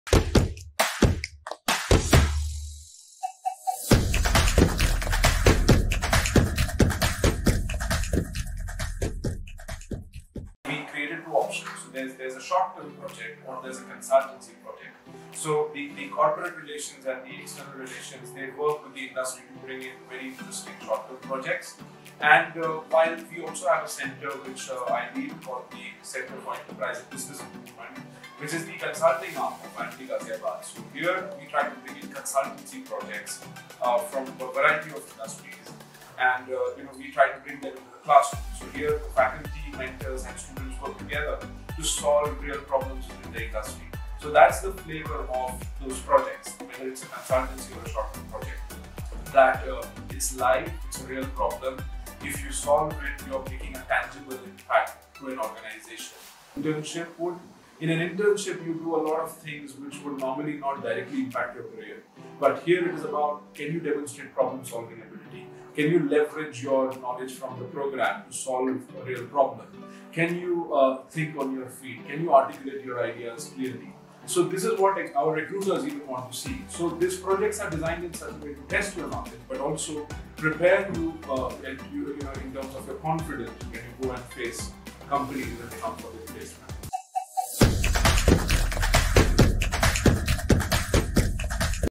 Learn about the Accelerated PGDM ExP program at IMT Ghaziabad | FireSideChat with Dr. Nihar Amoncar
The 18 months Accelerated PGDM for experienced professionals focuses on experiential learning by embedding ‘live’ consultancy and short-term projects which will challenge you to continuously test your knowledge.

Check out the FireSideChat with the Program Chairperson to learn more about the Accelerated PGDM ExP program at IMT Ghaziabad.

Applications are open for PGDM ExP 2022 intake!